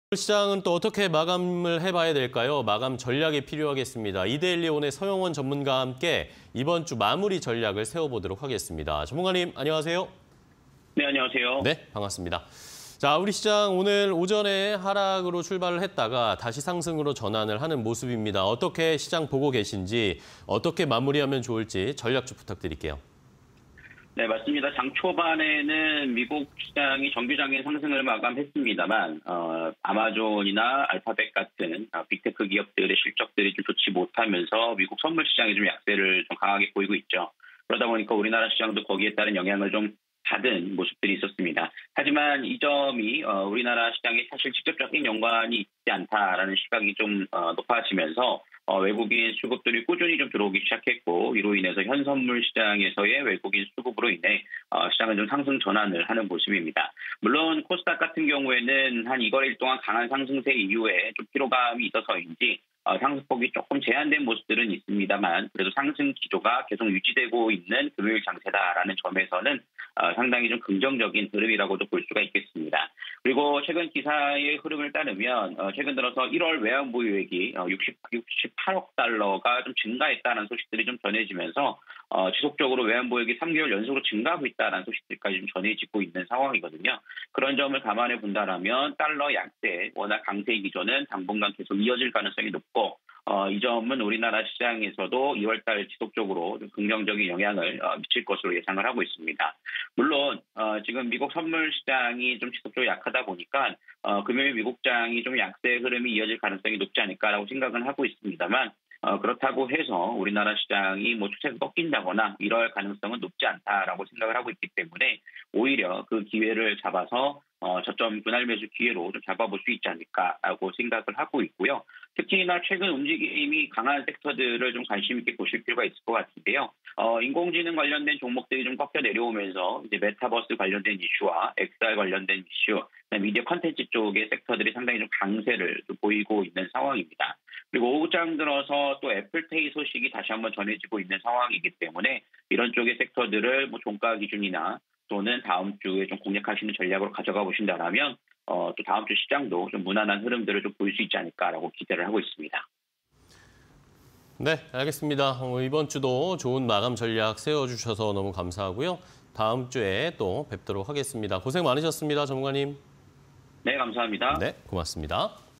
외국인 매수세 지속… 외환보유액 증가_서용원 마감 전략 (20230203)
- 외국인 매수세 지속… 외환보유액 증가
- 美 아마존ㆍ알파벳 실적 예상치 하회… 약세 예상
- 메타버스ㆍXRㆍ미디어ㆍ콘텐츠 섹터 강세